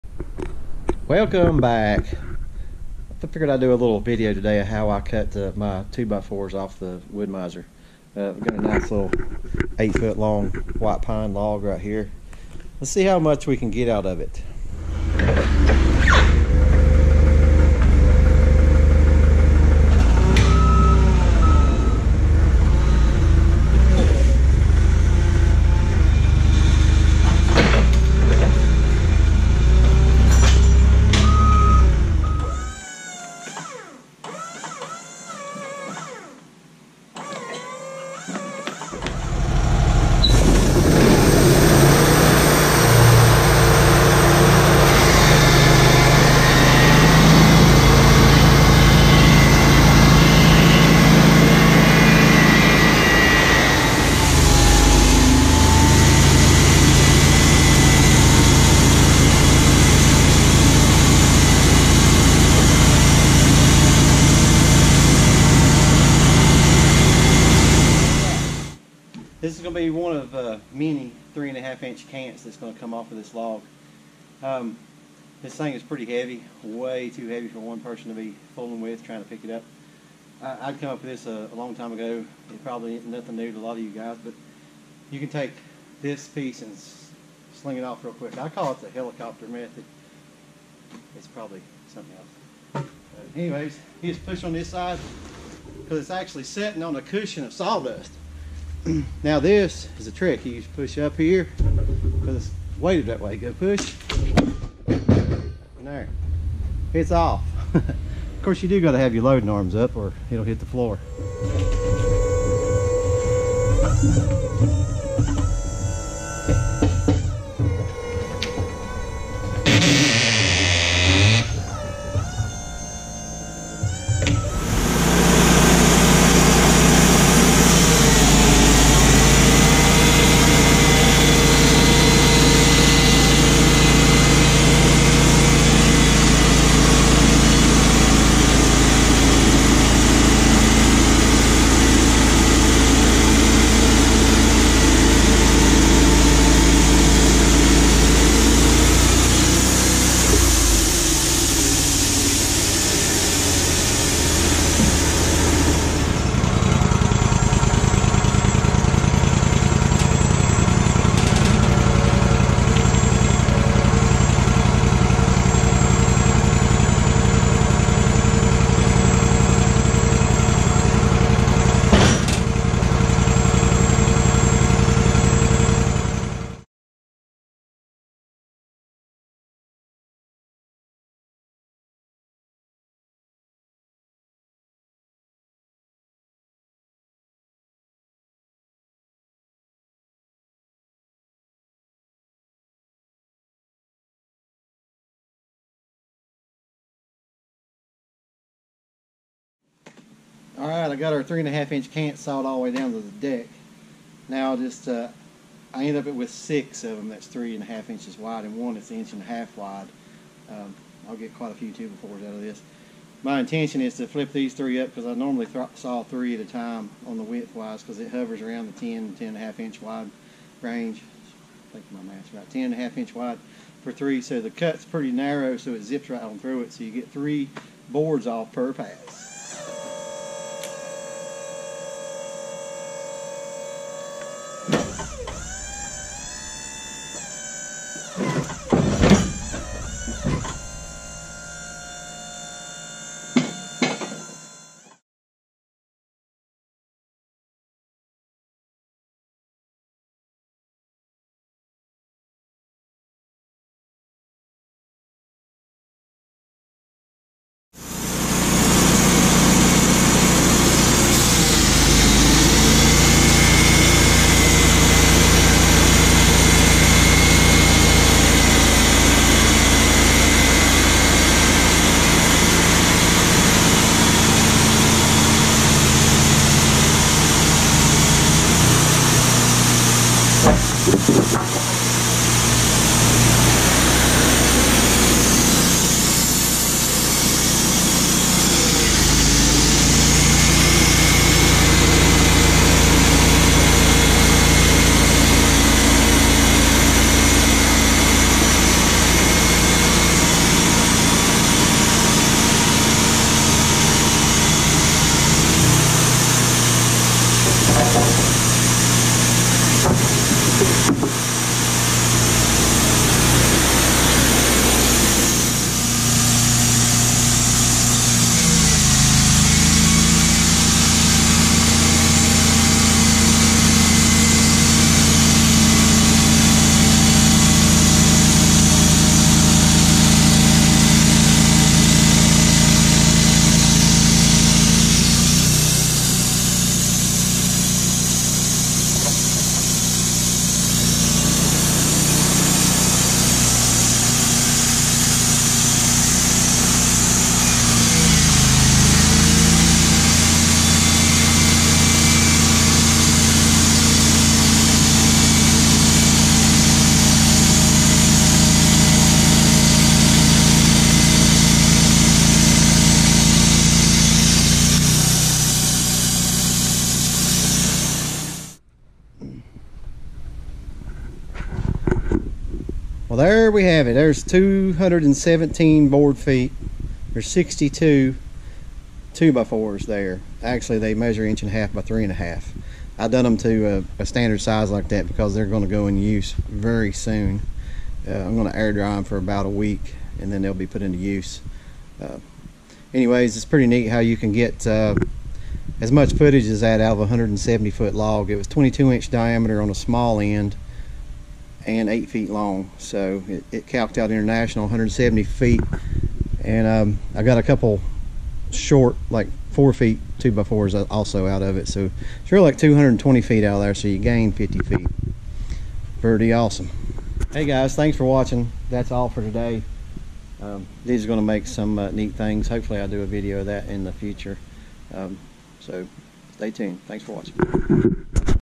Getting maximum footage from logs, sawing 2x4s on my woodmizer lt35. Oversaw
I demonstrate my method of sawing 2x4s on my lt35hd. Watch to the end to see how much footage I get from this 170 board foot (international) white pine log.